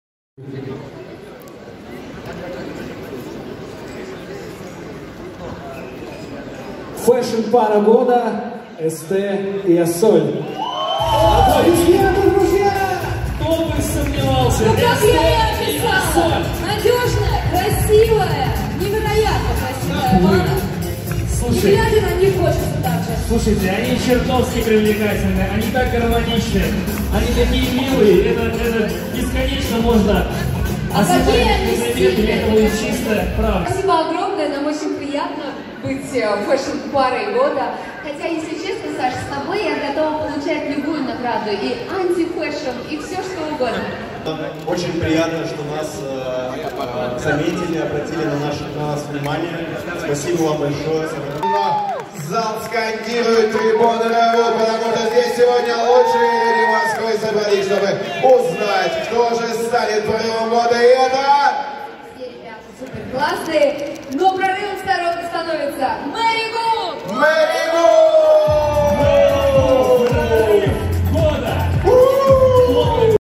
Рэпер ST привел на закрытую церемонию глубоко-беременную жену
Появление звездной пары на светской тусовке, конечно, наделало немало шума. В кулуарах уже давно обсуждали, что Ассоль, супруга артиста, ждёт ребёнка, но на мероприятиях её в последнее время никто не видел. Но тут пара сделала исключение и пришла получить награду на церемонии модной премии, да ещё и сама поучаствовала в награждении коллег. При этом Ассоль не побоялась обуть стильные сапоги на высоченных каблуках, будучи с уже внушительным животом. А со сцены она призналась, что с таким мужем ей, как говорится, ничего не страшно. Смотрите появление звёздной семьи за несколько недель до родов.